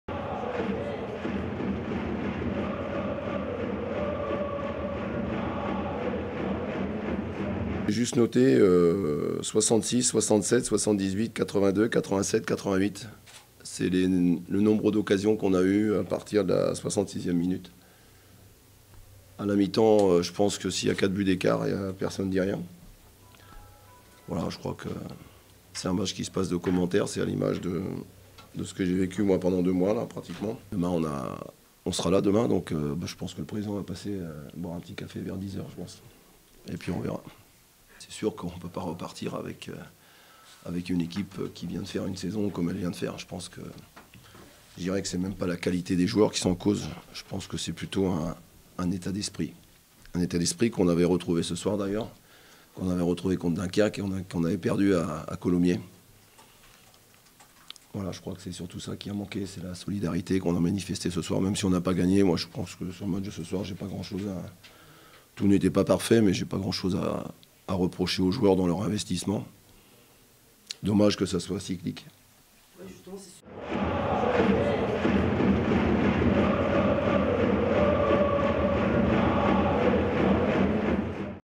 RCSA-Red Star (2-2) : les réactions l RC Strasbourg Alsace
Réactions de Jacky Duguépéroux, après le match nul du RC Strasbourg Alsace contre le Red Star.